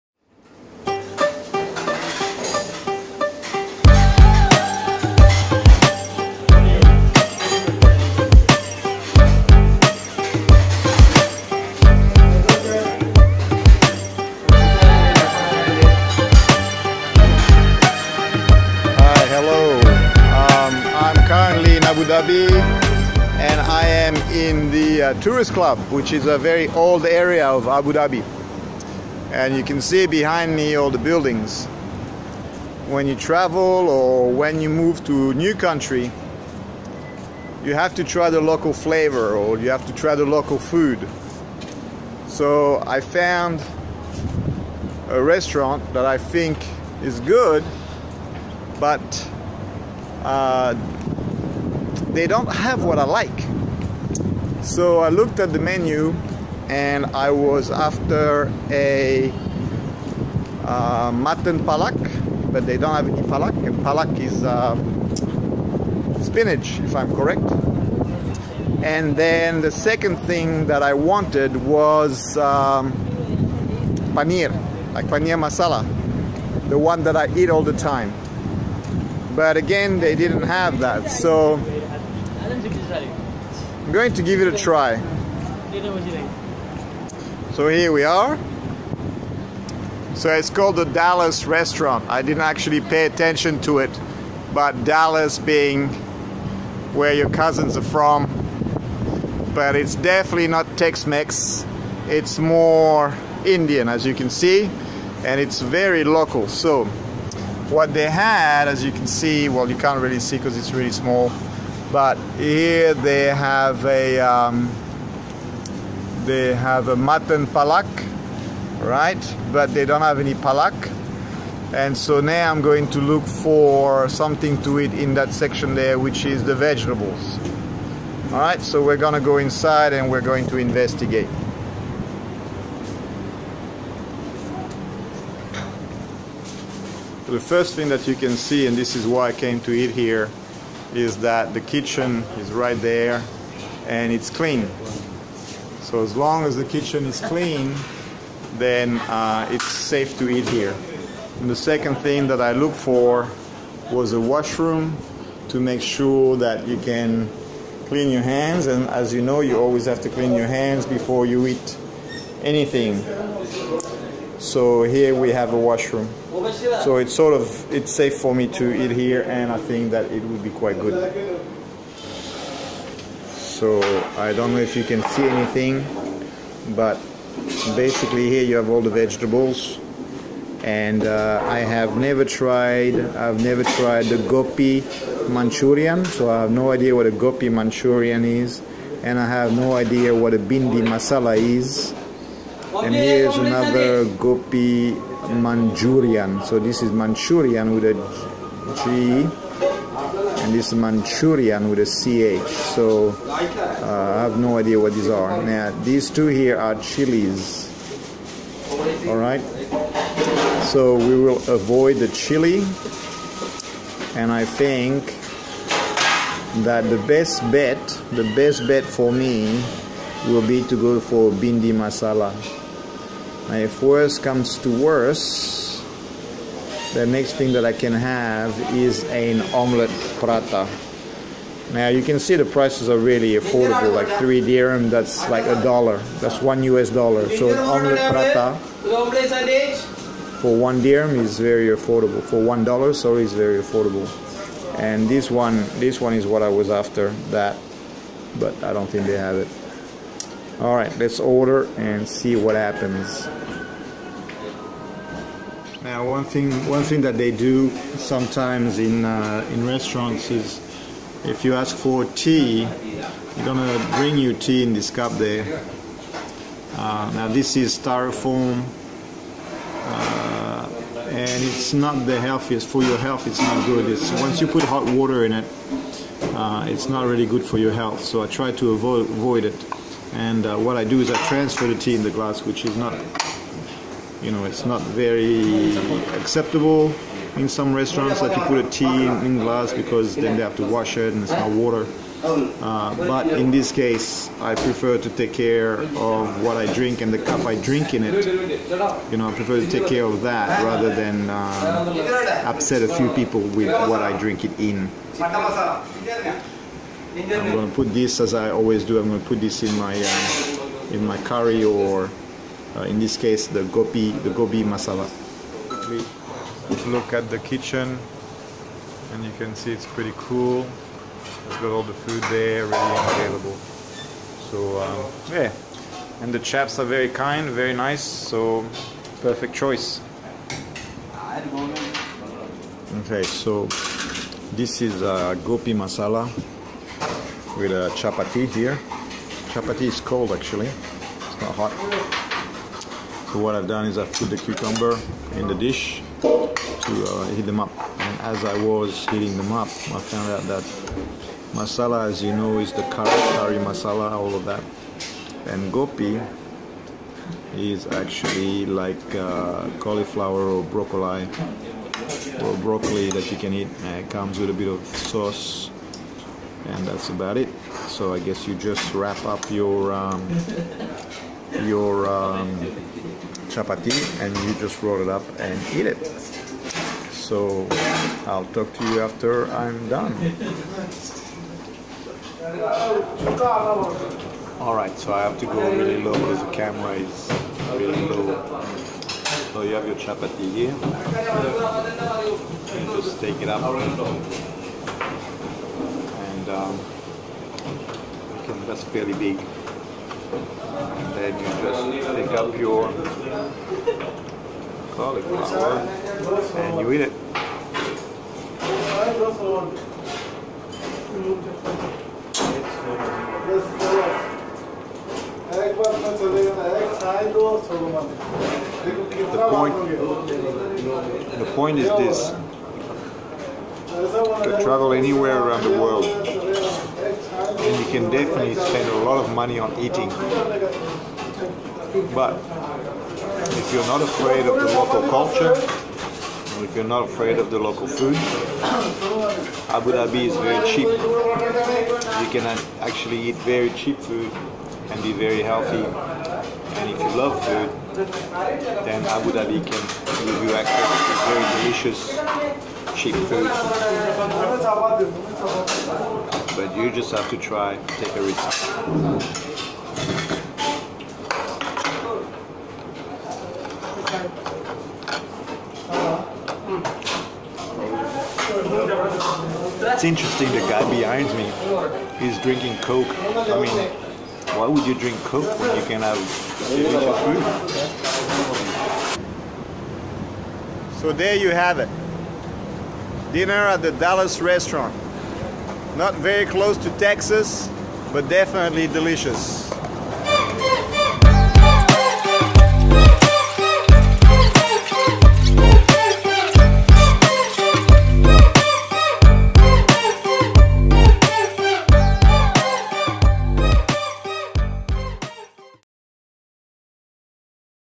Dallas@AbuDhabi
Dallas Restaurant is located in the Tourist Club area of Abu Dhabi. It is a clean and friendly restaurant with great delicious simple dishes to chose from.